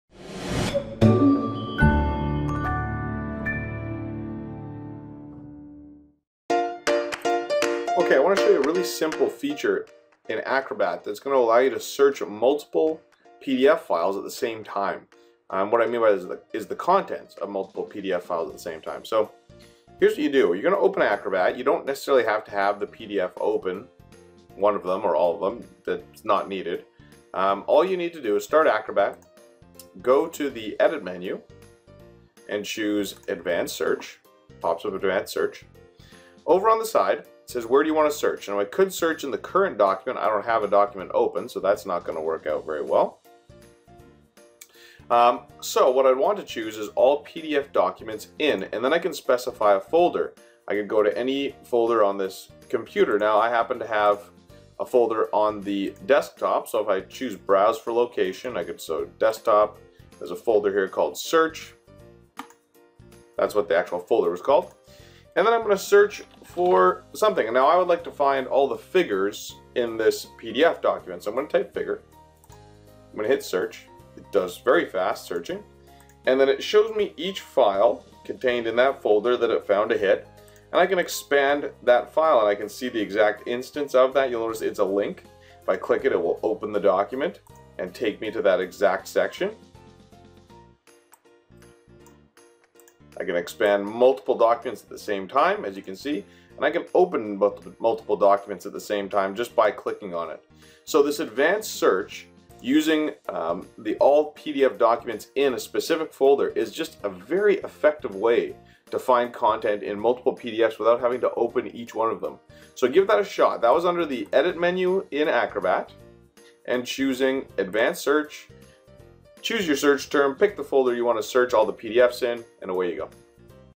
Adobe Acrobat - Search Multiple PDFs
This short video will show you how to use the advanced find feature in acrobat to search through multiple pdfs contained in the same folder.